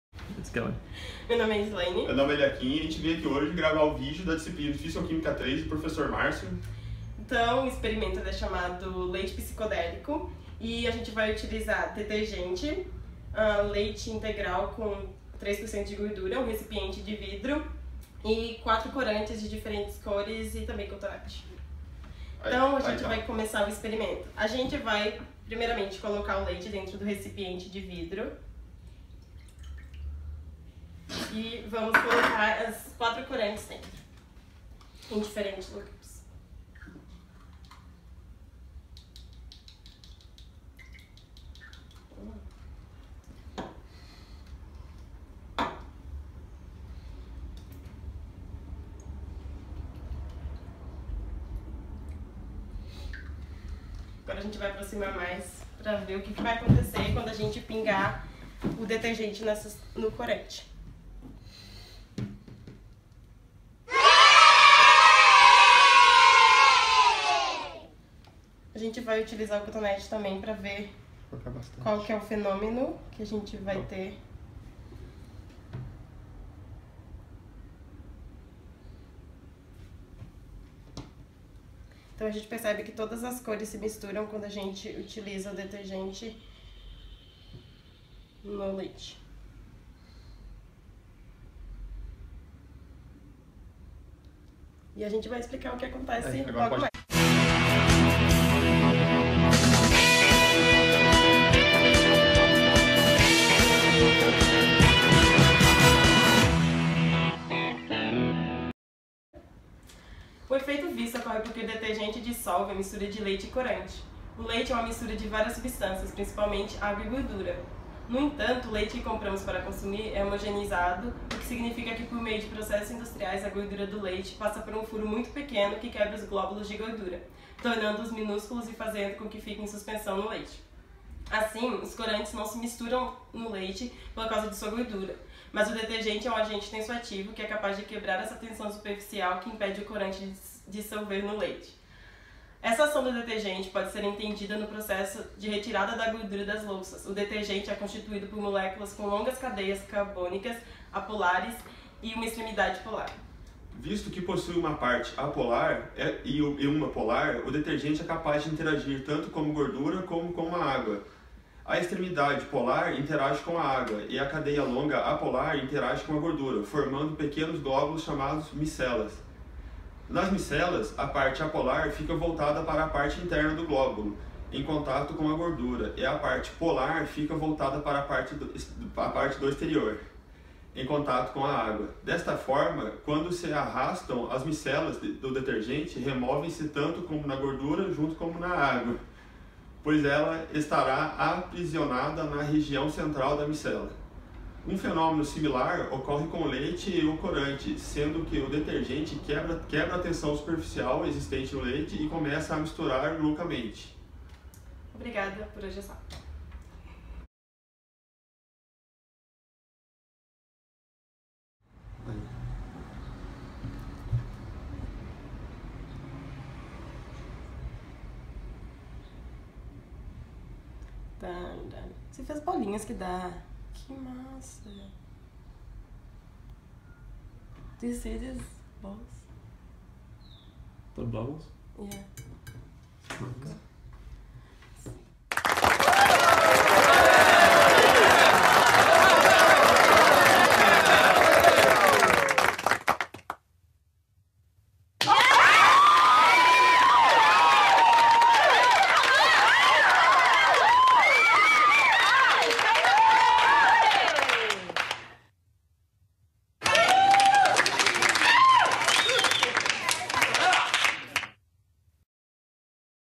Leite Psicodélico
Experimento de tensão superficial com leite, corantes alimentícios e detergente líquido.
Autores: Islayne de Paula e Eliakim Prado